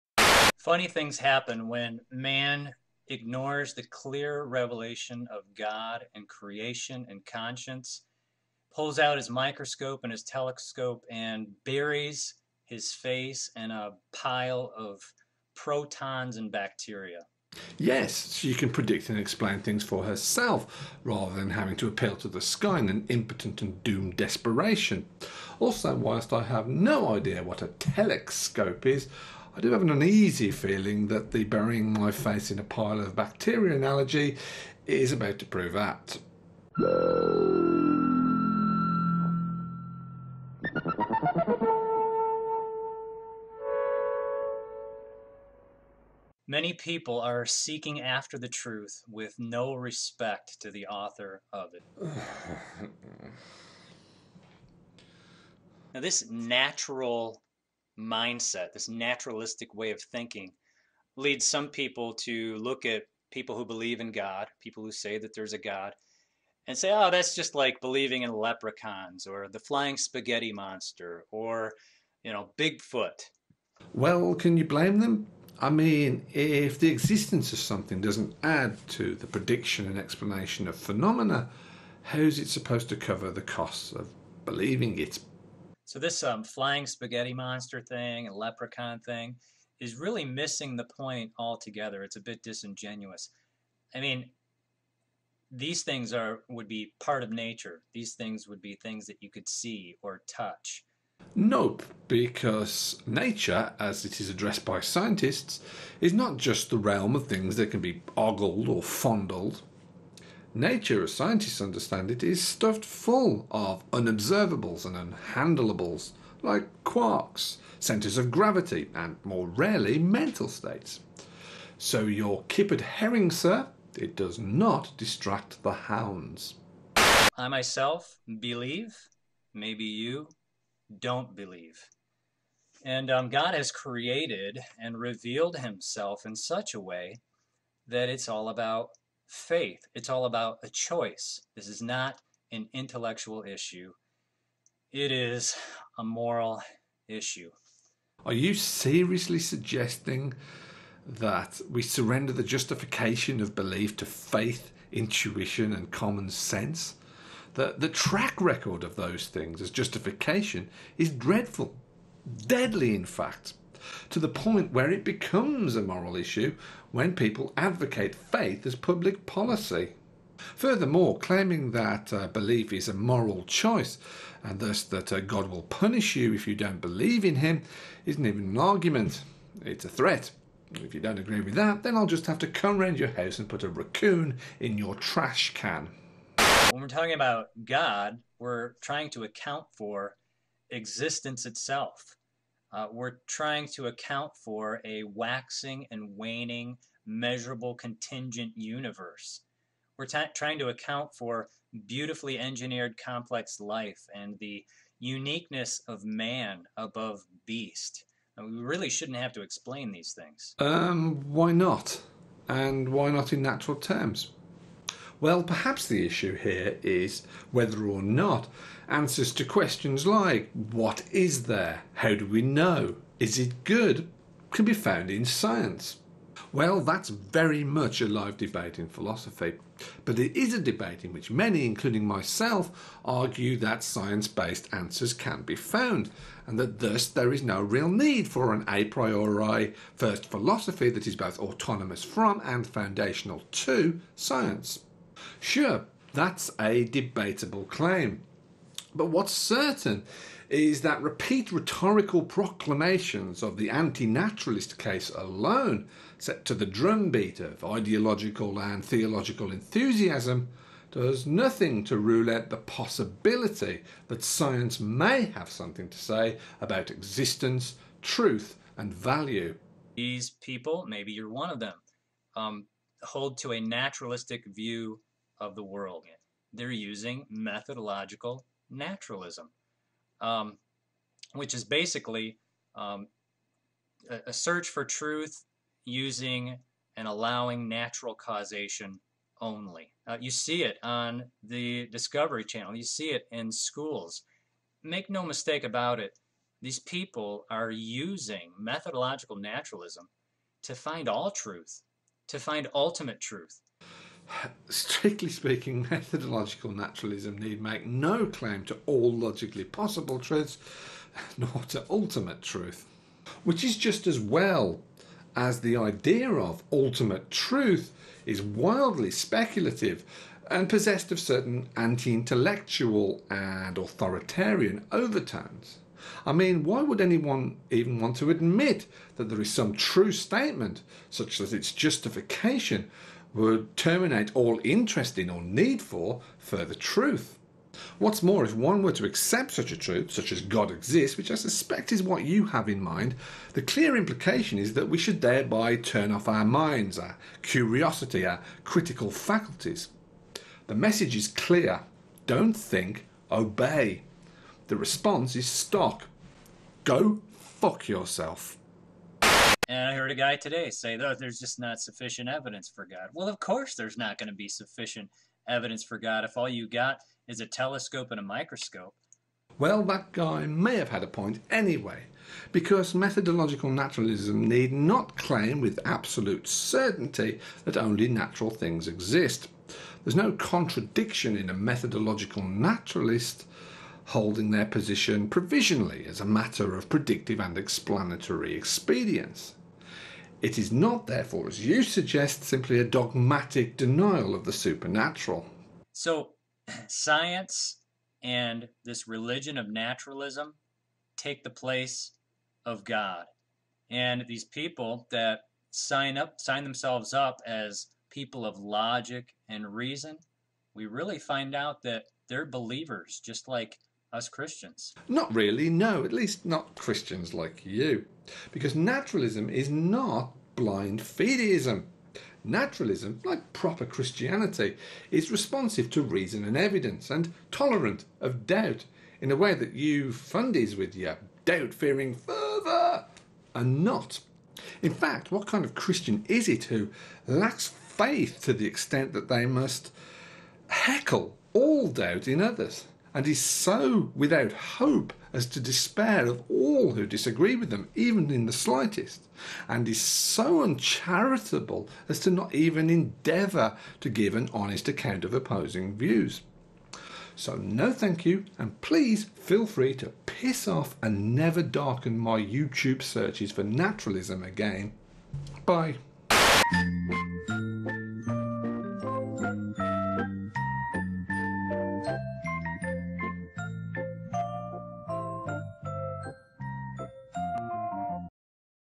Getting Nature Wrong
This was rendered by monkeys with pencils; either that or my video software has gone a bit wonky. Here's his original (again)

He's still talking the usual crap about naturalism...